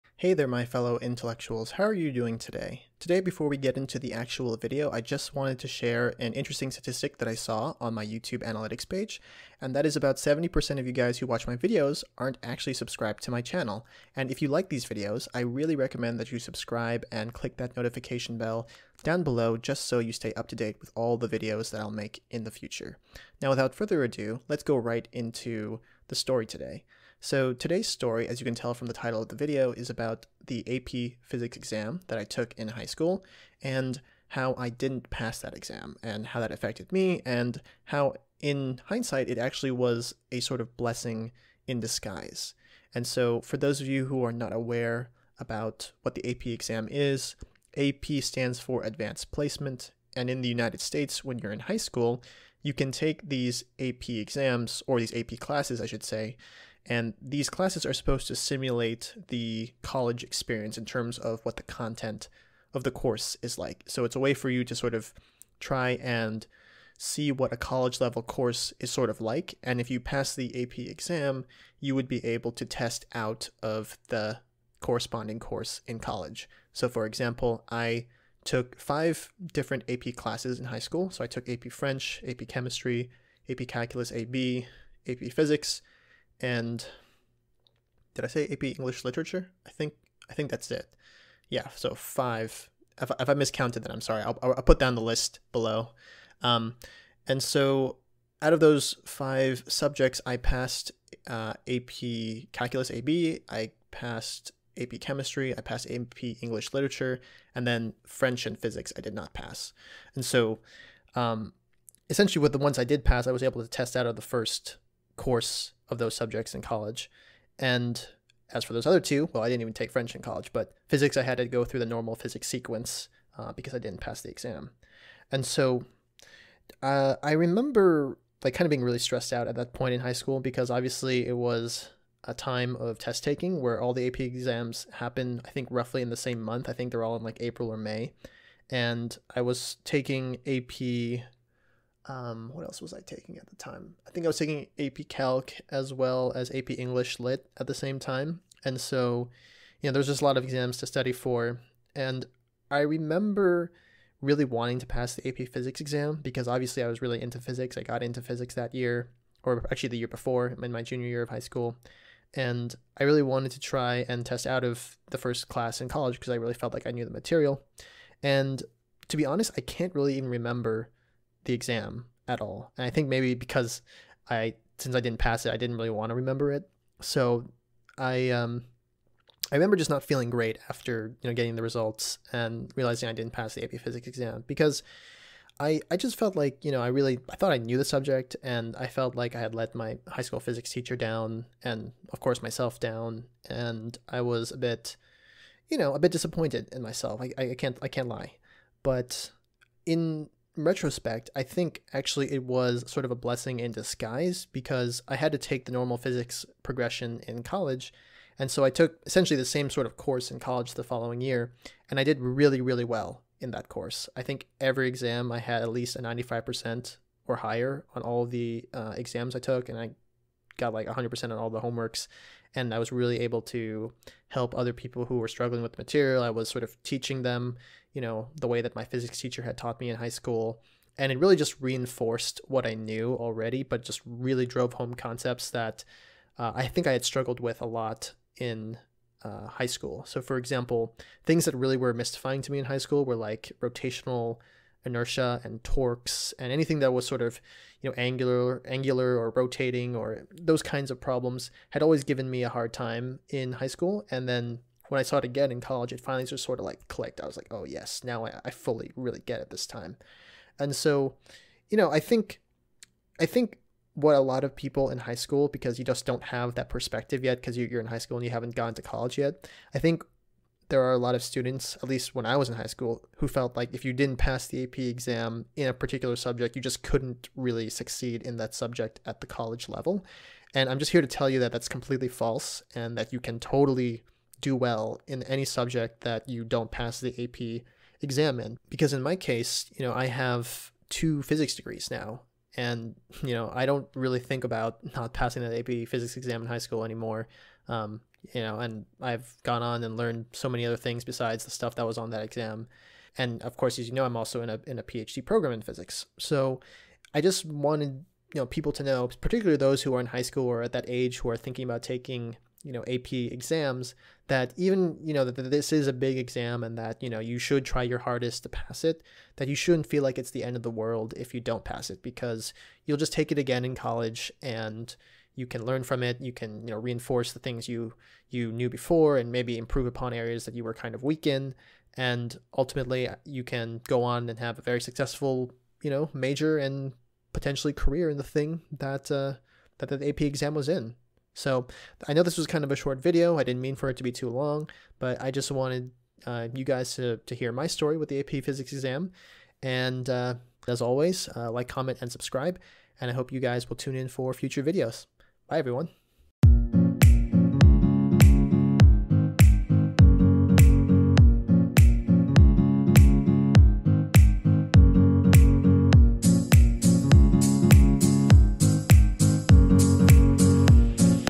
Story Time | Failing the AP Physics Exam in High School and How I Learned From Failure
In this video, I talk about failing the AP Physics Exam in High School, and how that experience helped me get better at physics in college and ultimately how failing an AP exam does not automatically mean you can't pursue that subject in college.